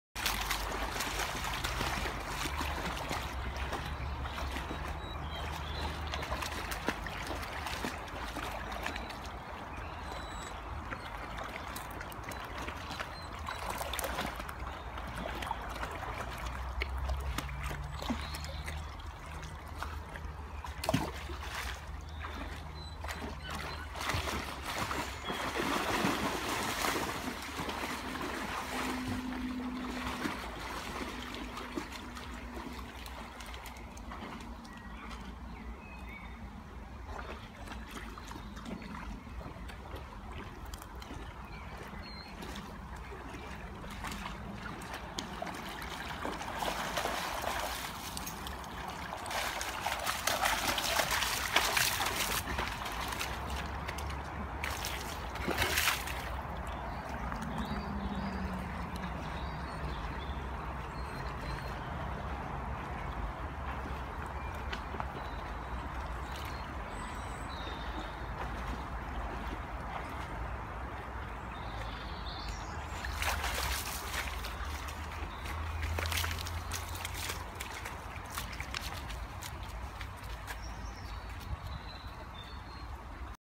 Komondor Cooling Down 2017
Young Komondor Rasta and Quinn cooling down in the water after a surprisingly warm day in Ireland, featuring Red the Mudi!
Video taken in 2017. Re uploaded after account restart